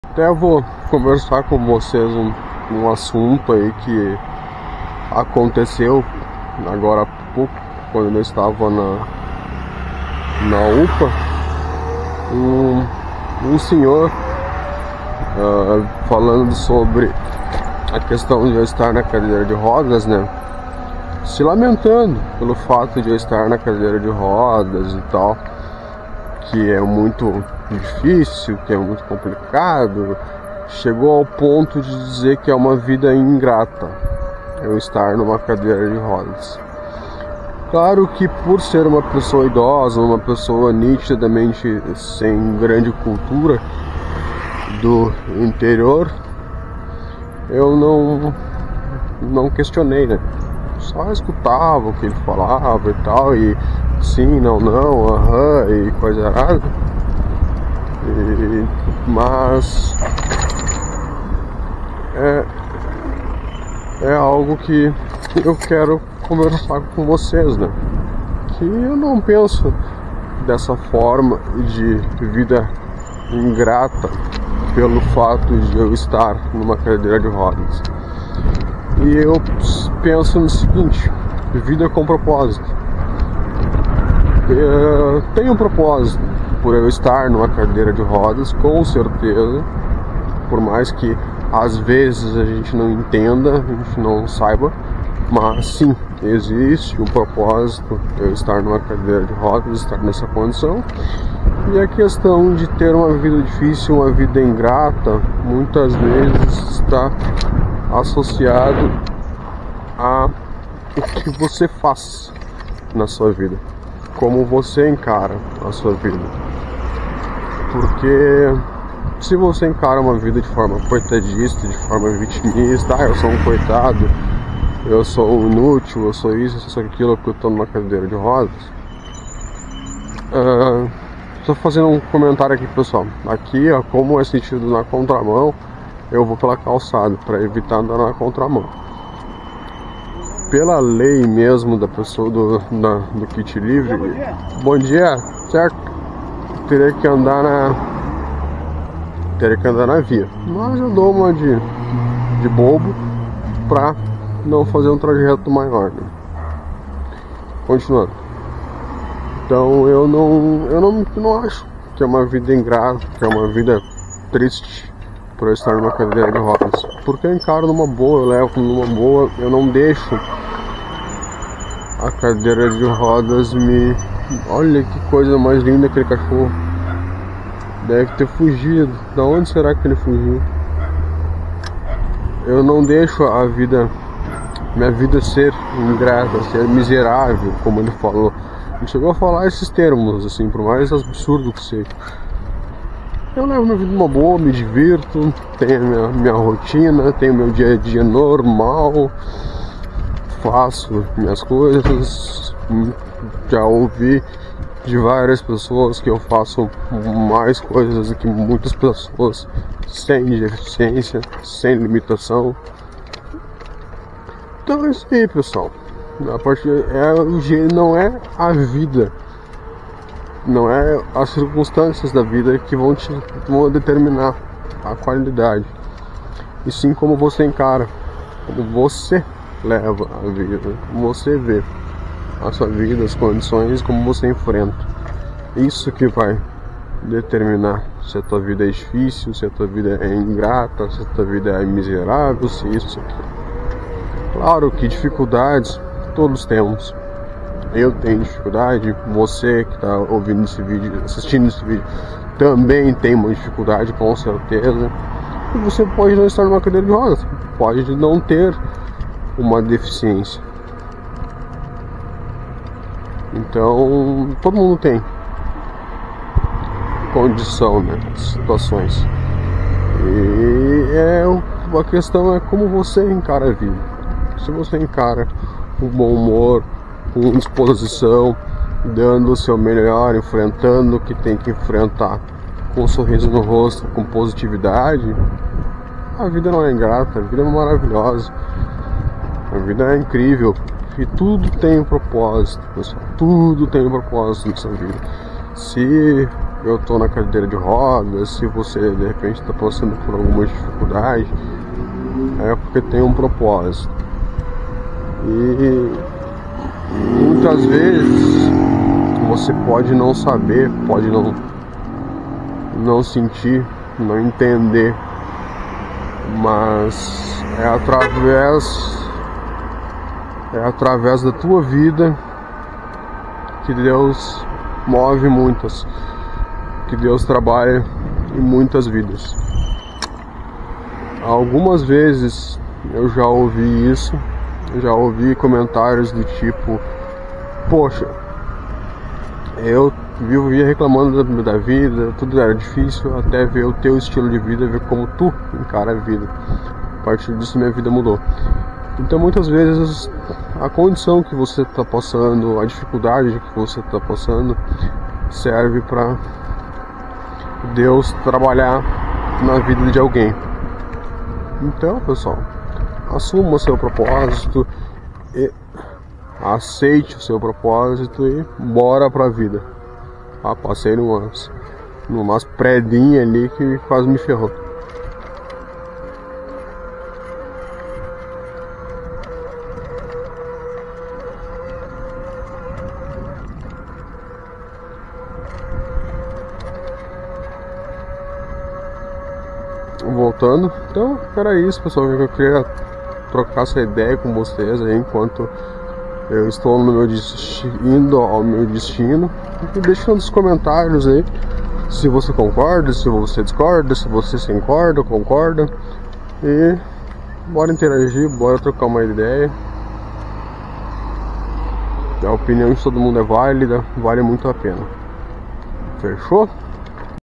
Vida miserável na cadeira de rodas?
Muitas pessoas tem o pensamentos que a vida em uma cadeira de rodas é difícil, complicada, triste e até miserável.
Eu discordo totalmente, pois na minha opinião o que vai determinar sua vida não é uma cadeira de rodas e sim a forma que você enxerga a vida, como você enxerga a si mesmo e encara seu dia a dia.
Eu tento enfrentar da melhor forma possível, tentando sempre ter pensamentos, ações e palavras positivas para que tudo se torne mais leve. Sempre brinco que não sofro bullying pois eu mesmo o faço comigo mesmo.